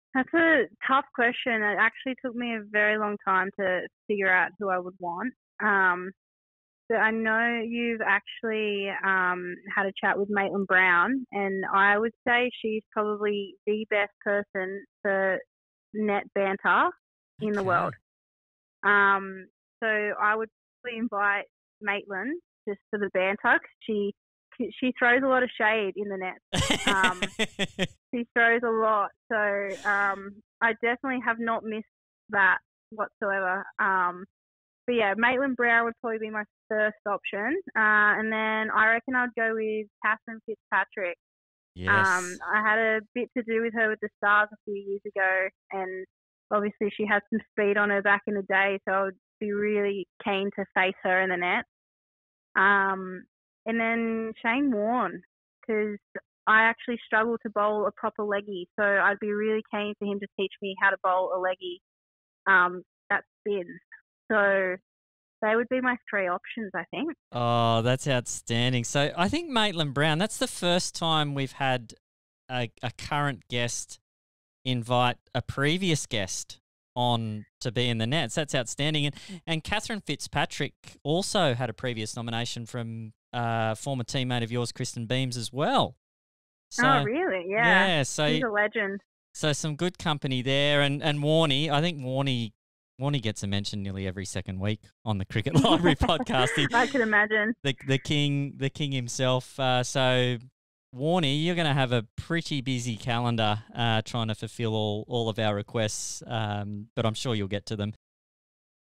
Maddie Penna's Dream Net Session
Maddie Penna selected her three guests for a dream net session on the Cricket Library Podcast.